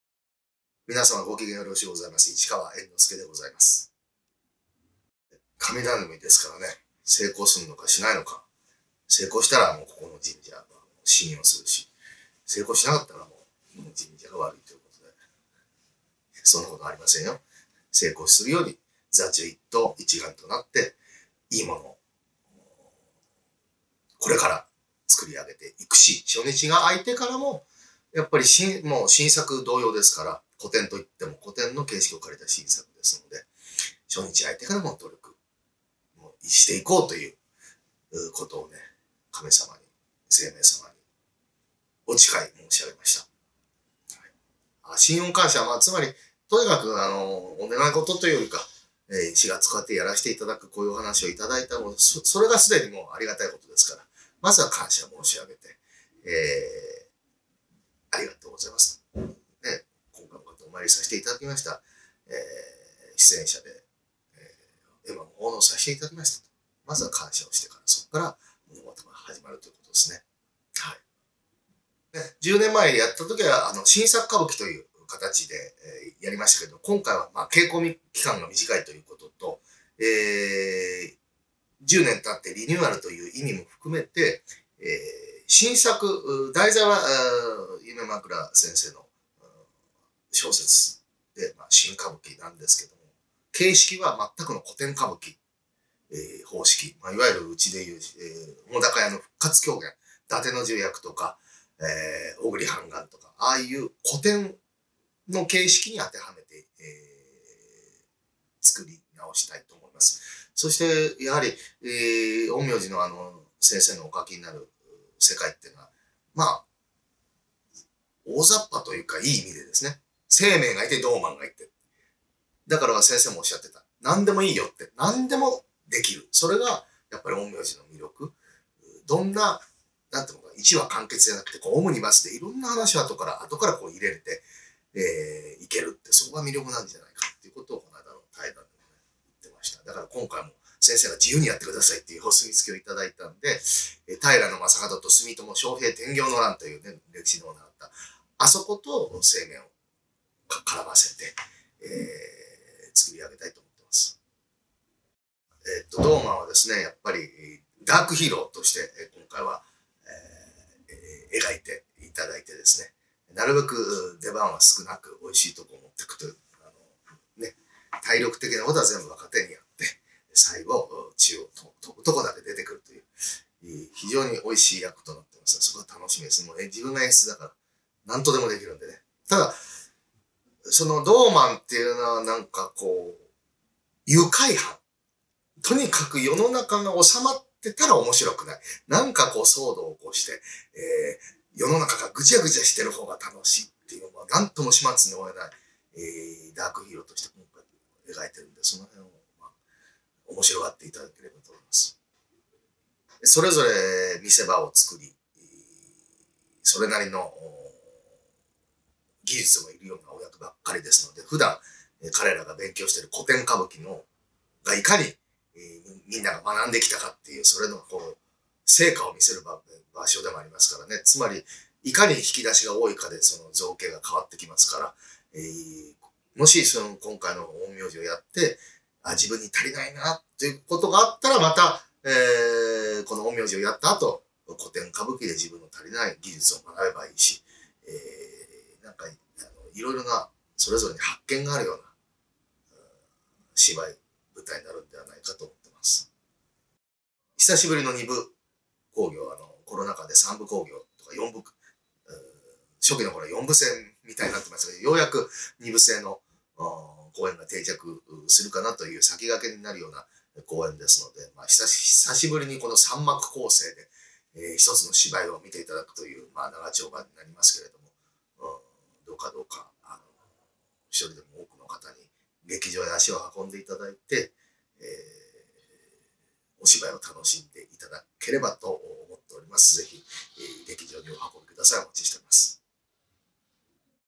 【歌舞伎座】「鳳凰祭四月大歌舞伎」昼の部『新・陰陽師』市川猿之助コメント映像
歌舞伎座新開場十周年記念「鳳凰祭四月大歌舞伎」
昼の部『新・陰陽師』

4月2日（日）初日→4月27日（木）千穐楽